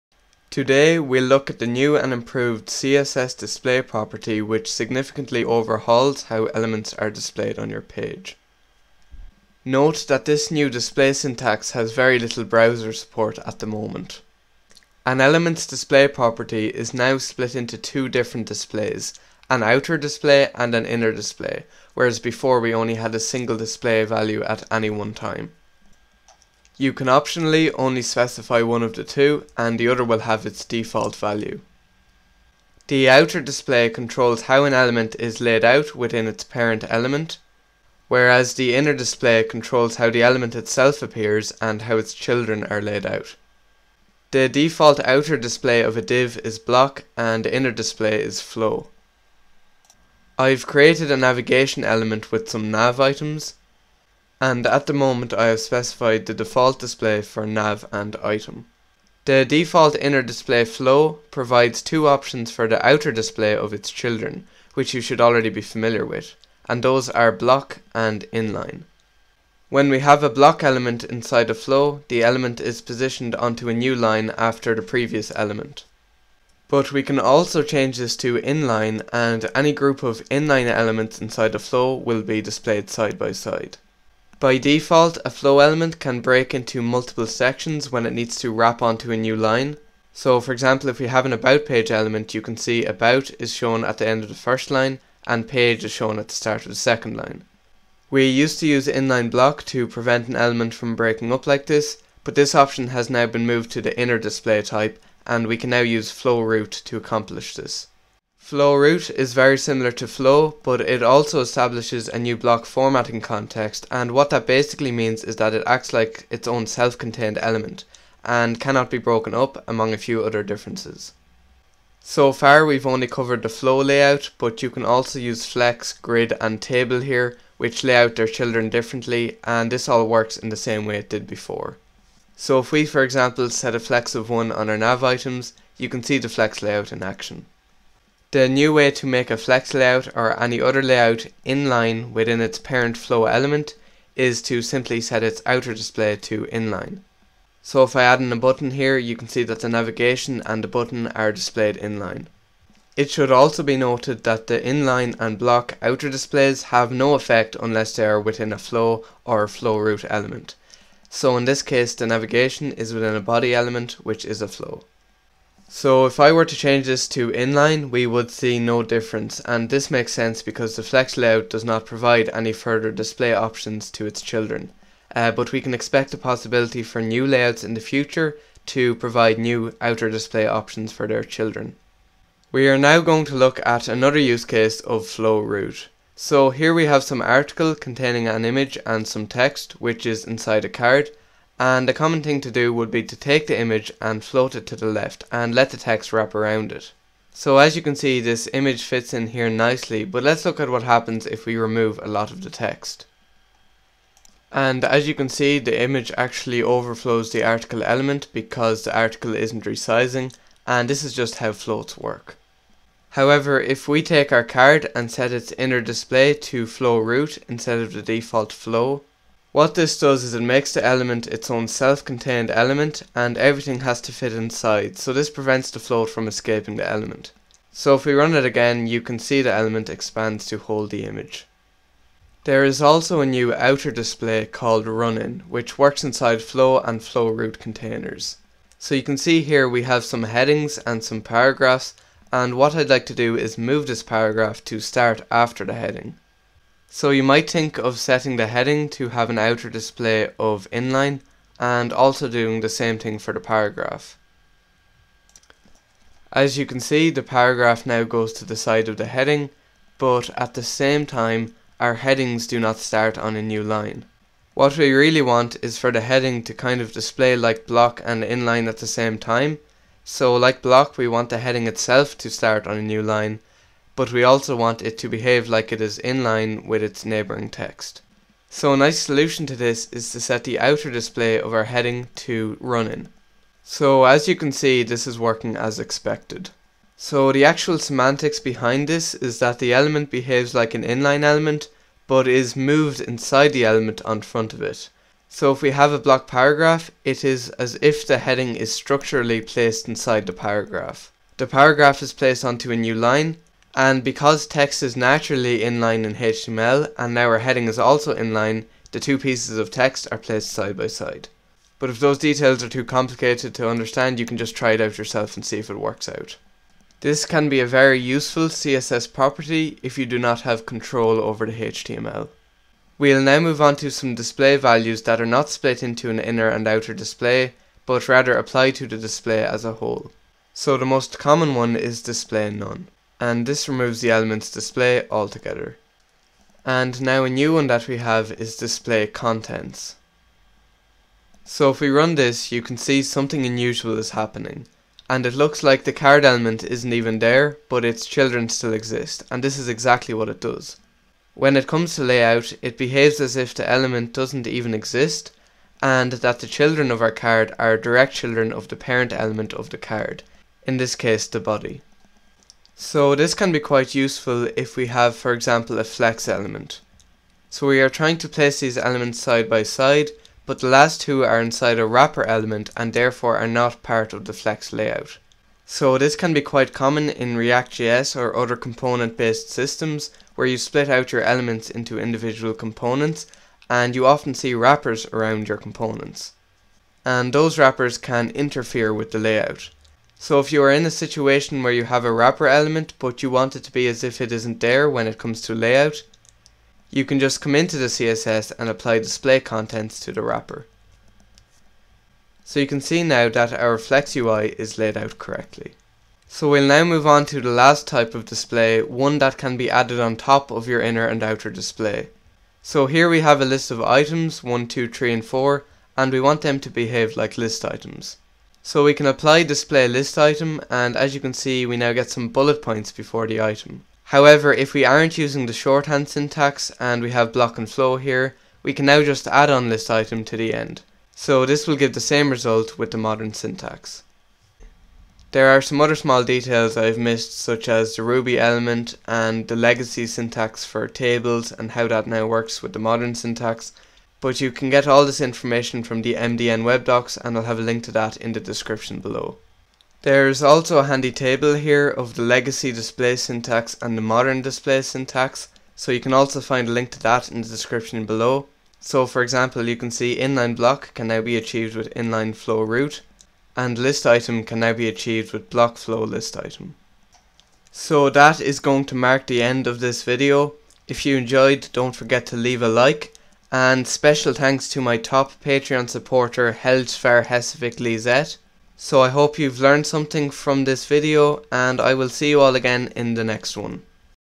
Everything you need to know about the NEW CSS DISPLAY syntax and features.
The CSS display property has updated syntax and a few new values that you can use such as flow-root and run-in. Check this video out for all the details!